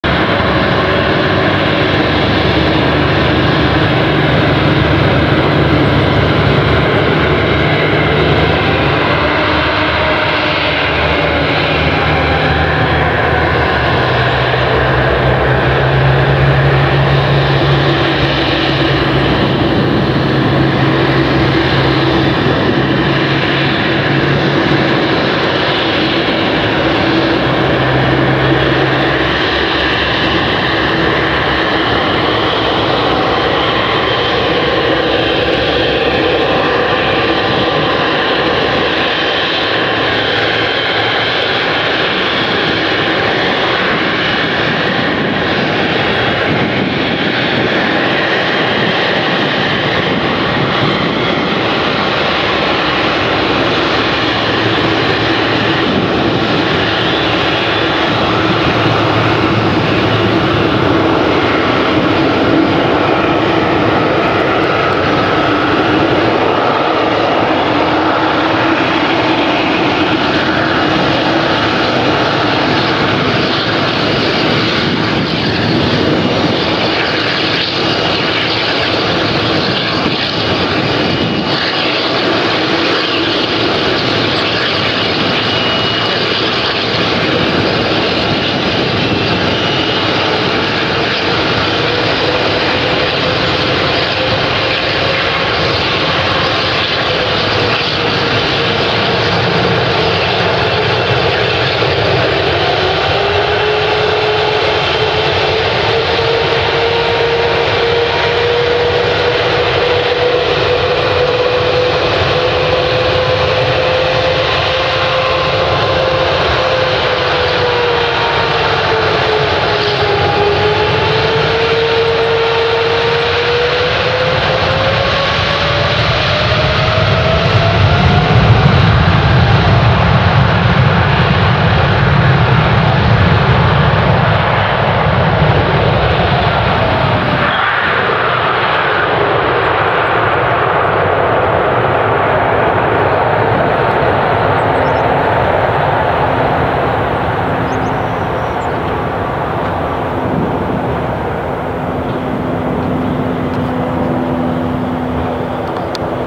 KLM 777 Taxi Test
PH-BVI taxi test before first flight at Paine Field June 28, 2012.